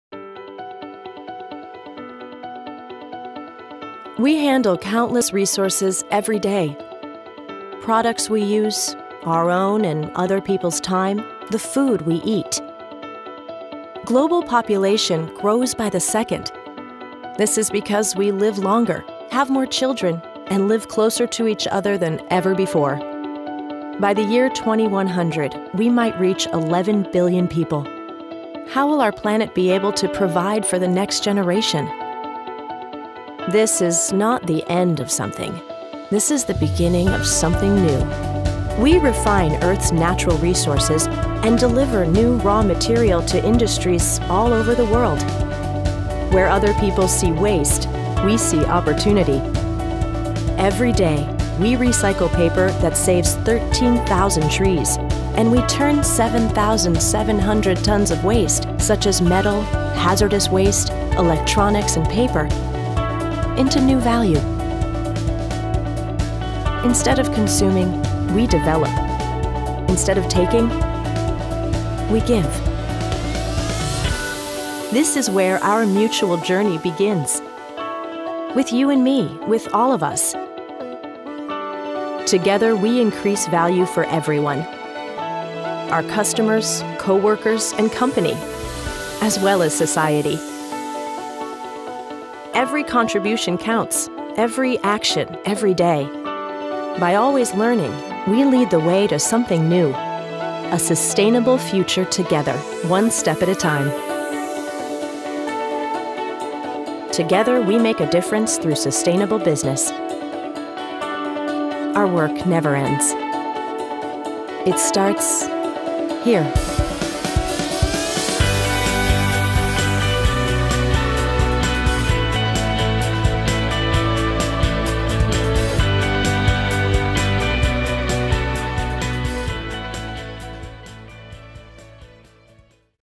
Stena Recycling - It Starts Here (Finnish subtitles)
We refine earth’s natural recources and deliver new raw material to industries all over the world. Where other people see waste, we see opportunity. Together we increase value for everyone. Together we make a difference through sustainable business. Our work never ends. It starts here.